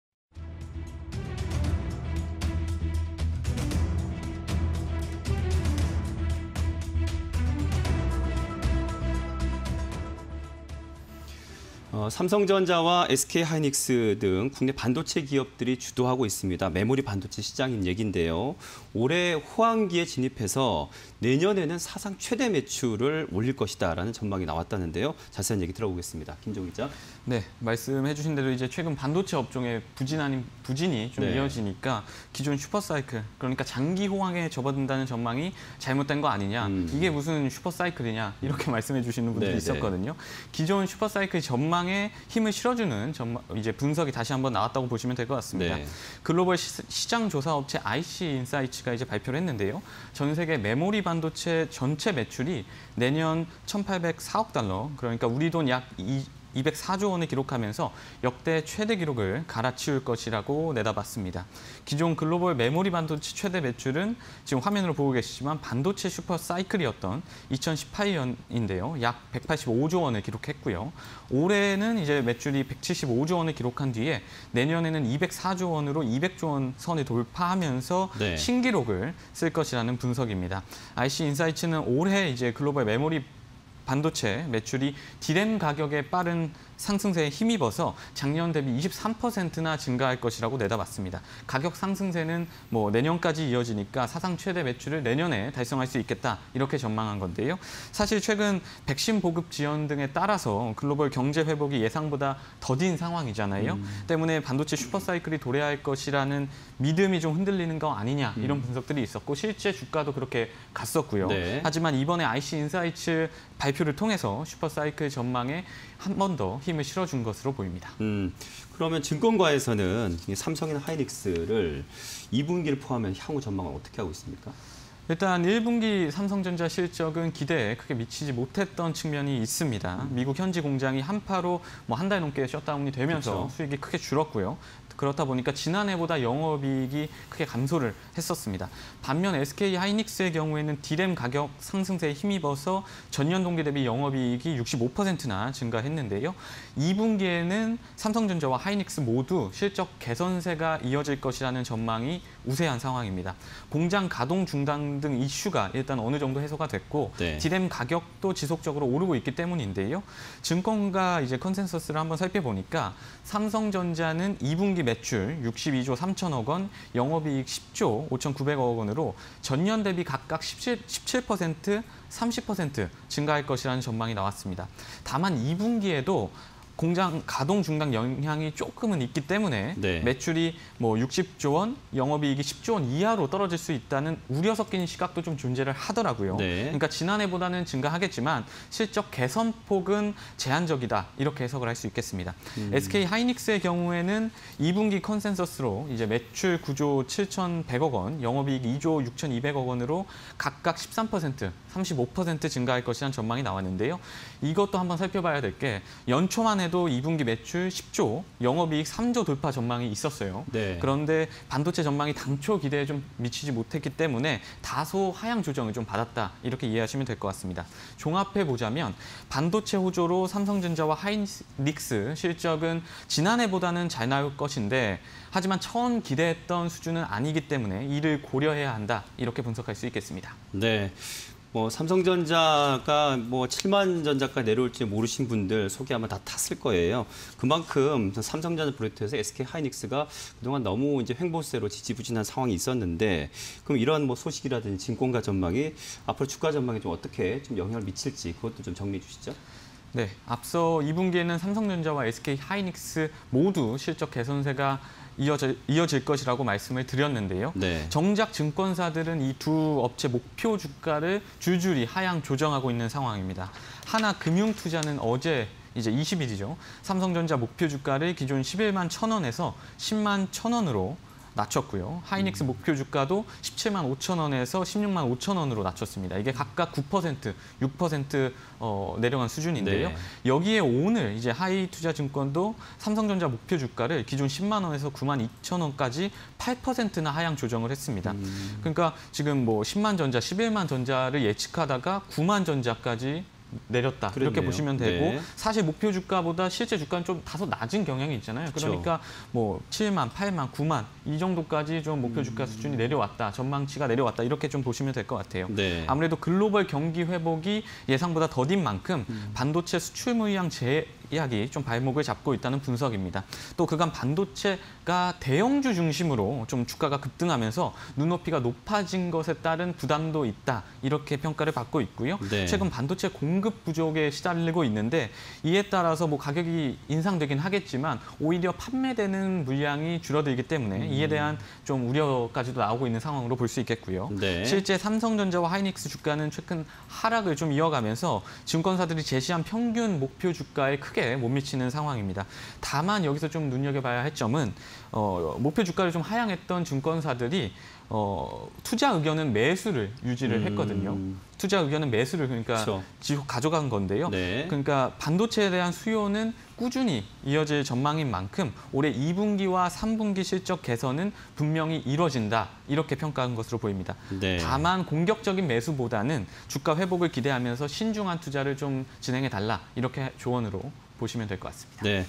메모리반도체 호황기… "내년 사상 최대 매출" 전망_뉴스IN이슈 (20210521)
- ‘반도체 슈퍼사이클’ 전망에 힘 실어
- 2분기 삼성전자·SK하이닉스 실적 개선 흐름
- “큰 폭 실적 개선은 어려워... 당초 목표 하회
- 증권사, 반도체주 목표 주가 줄하향 움직임
- “글로벌 경기 회복 더뎌... 판매량 제한적”